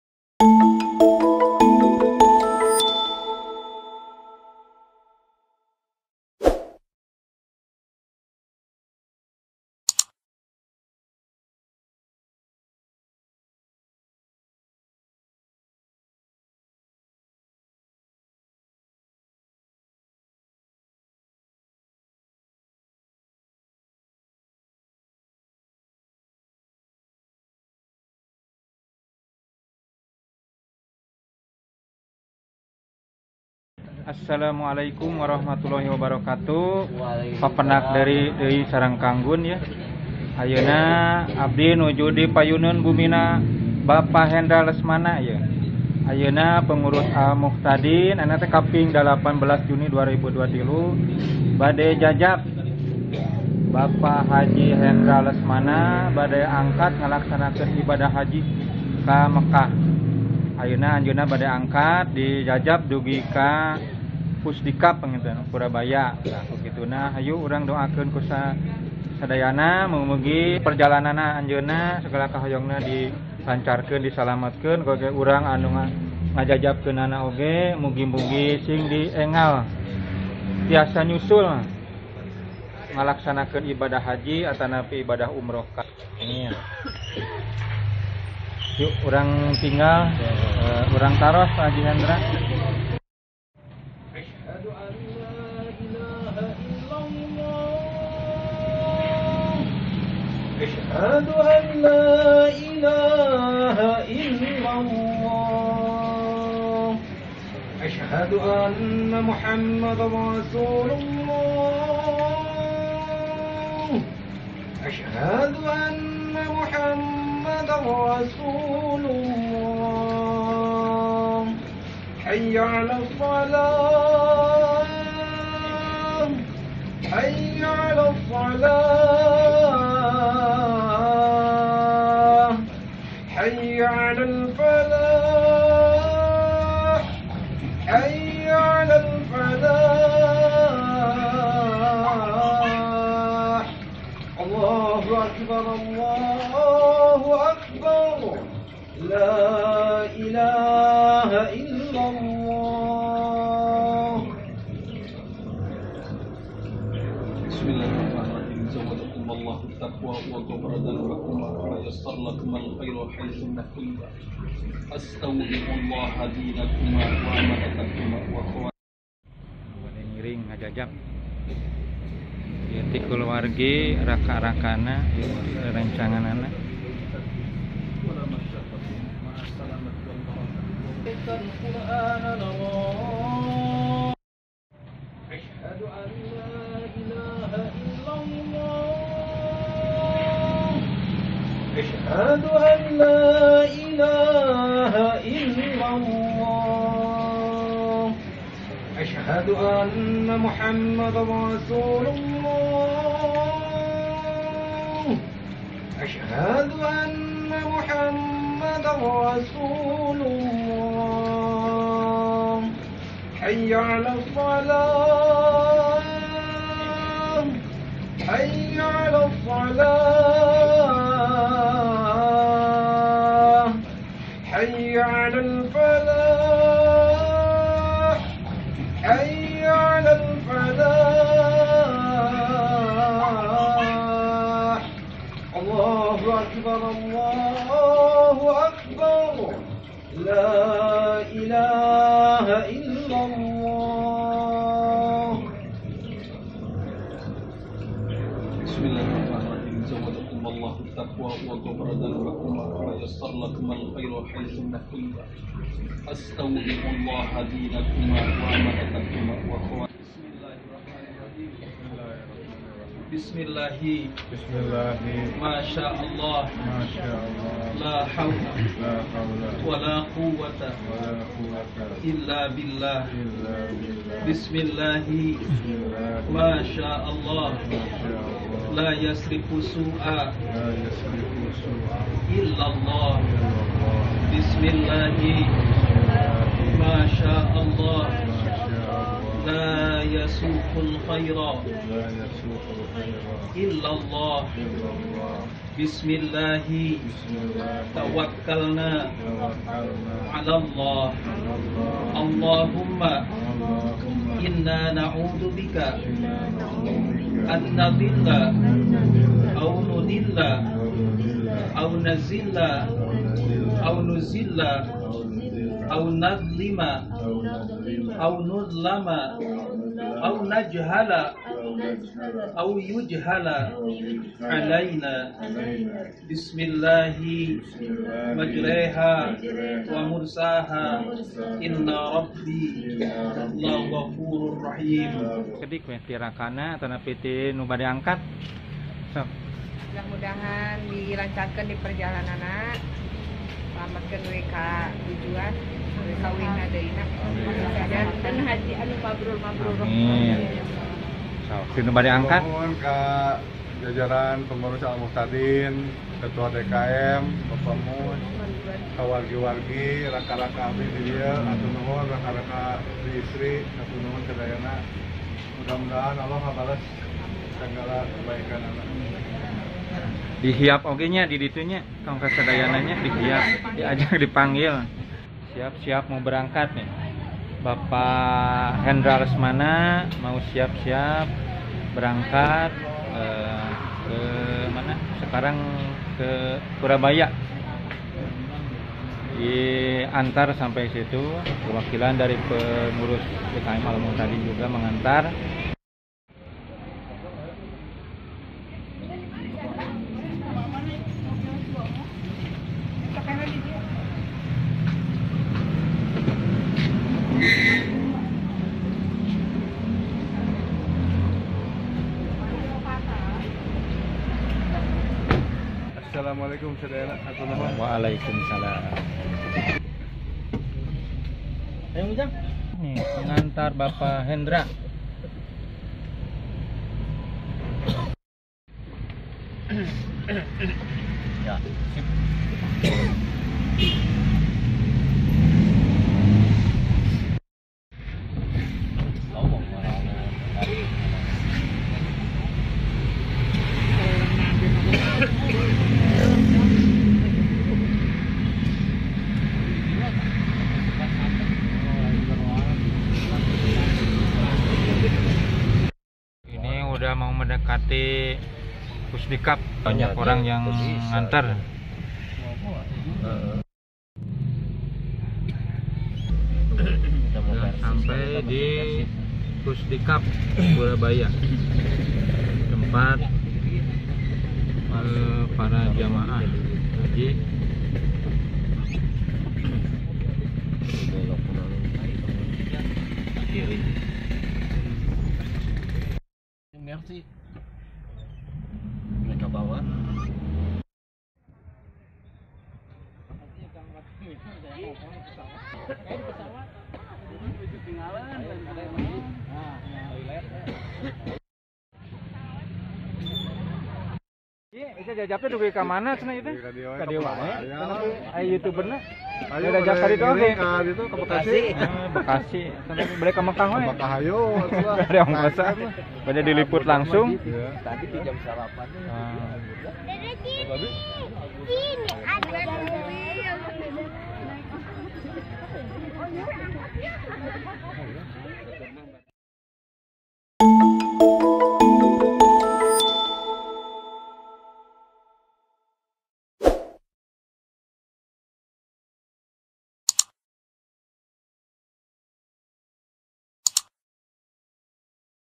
Mengantar Jemaah Haji 2023. 18 Juni 2023
Jemaah Haji asal Indonesia mulai diberangkatkan termasuk Jemaah Haji yg berasal dari Sukamaju RW 06 Desa Padalarang yaitu bpk. Hendra Lesmana sbg pengurus DKM  AlMuhtadin,pengurus lain dan keluarga mengantar keberangkatannya sampai PUSDIKKAV  Purabaya Padalarang.